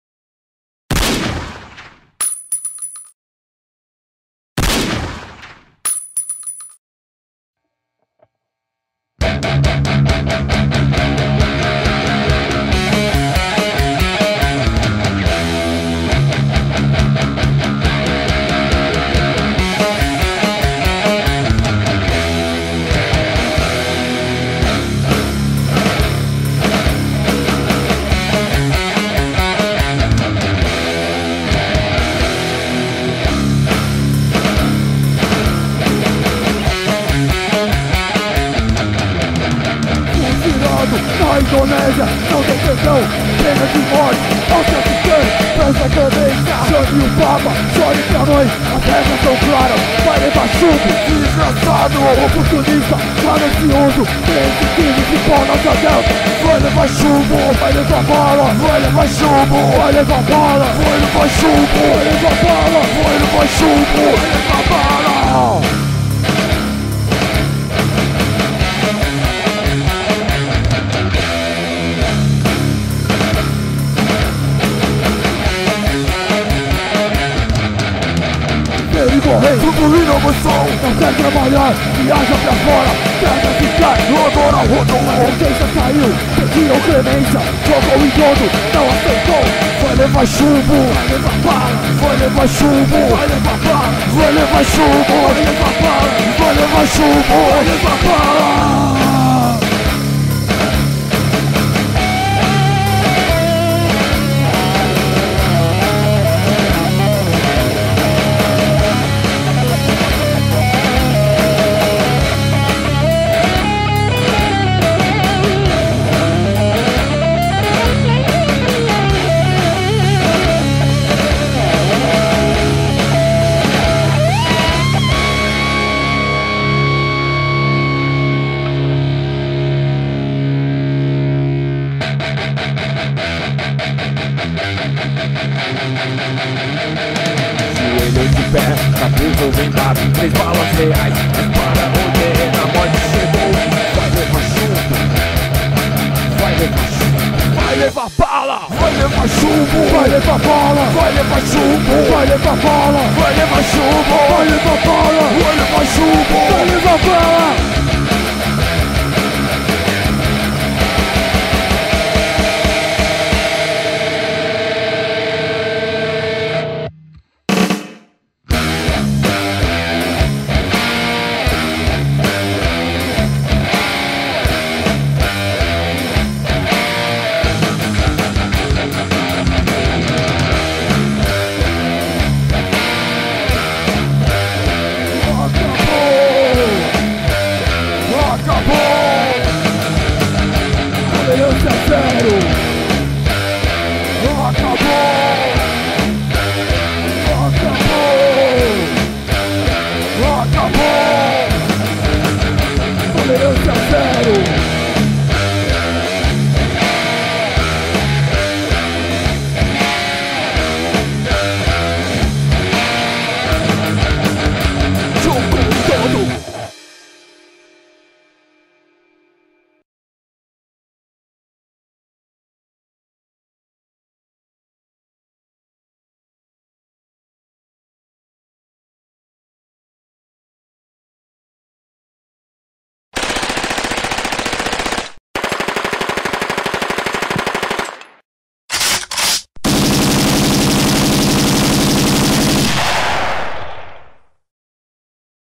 Fuzilado na Indonésia ( Hell Files )
Obra retratando a morte do Brasileiro por fuzilamento na Indonésia.
Indonésia: ''Alguns infratores causam danos irreparáveis na sociedade. Nossas leis são rígidas e nosso povo apoia. Continuaremos combatendo o tráfico de drogas com a pena de morte".
Brasil: ''Não houve sensibilidade por parte do governo da Indonésia para o pedido de clemência do governo brasileiro".